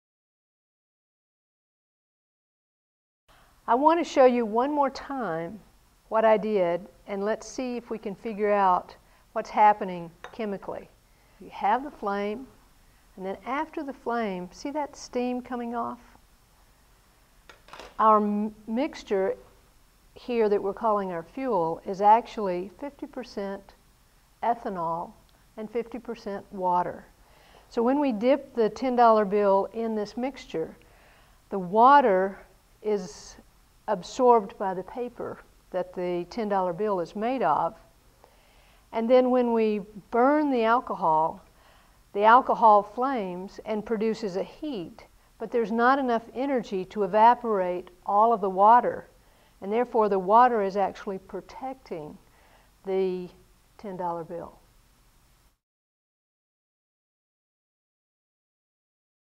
Burning $10 Bill Part 2
Part of NCSSM CORE collection: This video shows part 2 of the burning of a $10 bill demonstration.